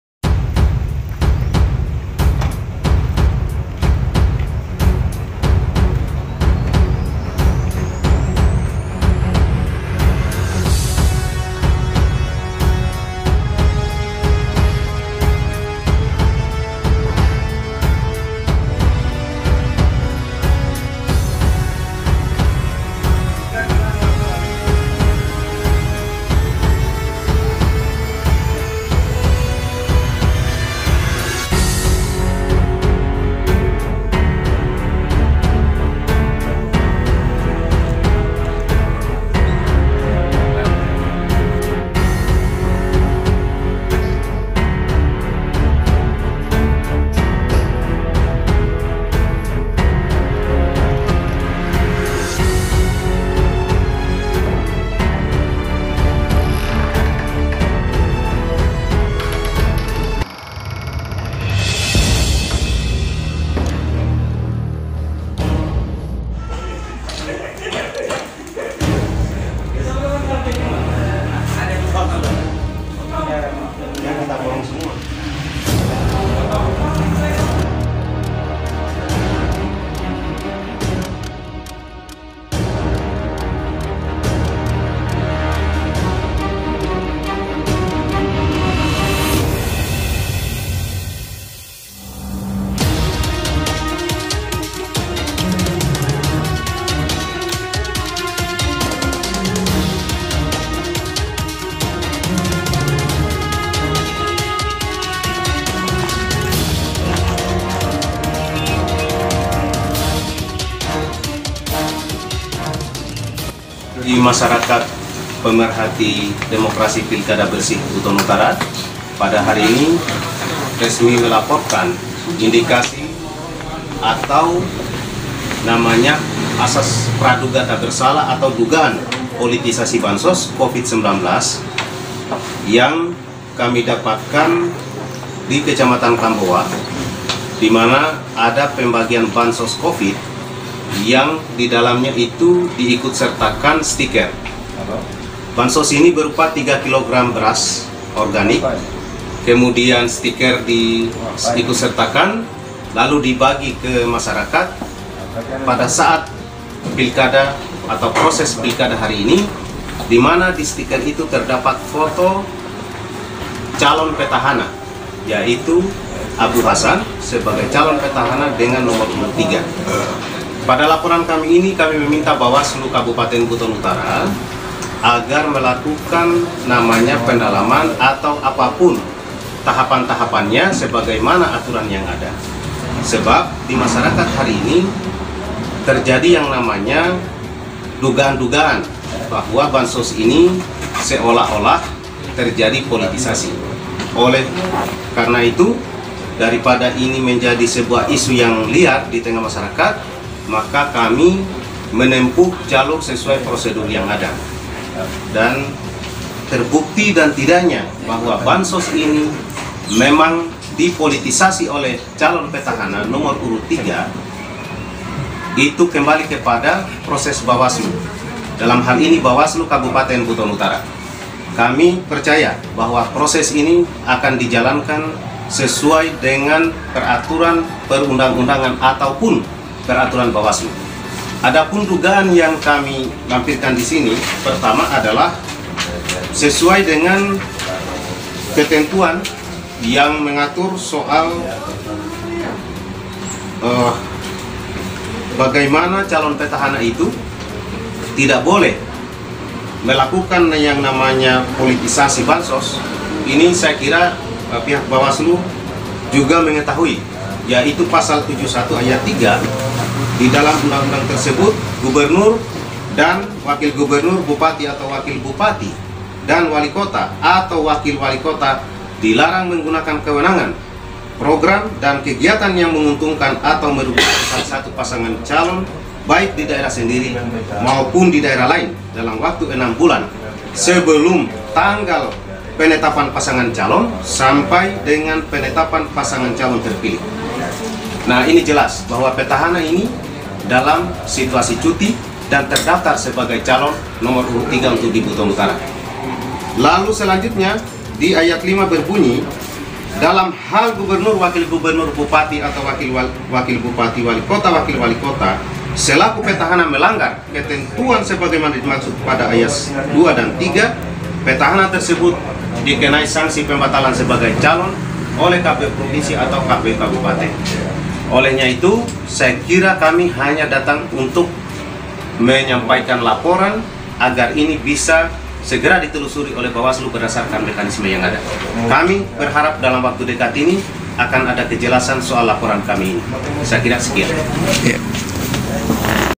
Cakada Petahana Buton Utara Dilapor Ke Bawaslu Indikasi Politisasi Bansos Covid-19, '2020
MSY bersama Masyarakat Pemerhati Demokrasi Perduli Pilkada Bersih Buton Utara Laporkan Indikasi Politisasi Bansos Covid-19 Di Bawaslu Buton Utara, Kami 26 November 2020.
covid-19
CHANNEL INI DIBUAT UNTUK MENJADI SALURAN SEGALA HAL YANG DIANGGAP SIFATNYA POSITIF DAN BERMANFAAT UNTUK DISEBARKAN

SEMOGA SEMUANYA SENANTIASA DIRAHMATI TUHAN YANG MAHA ESA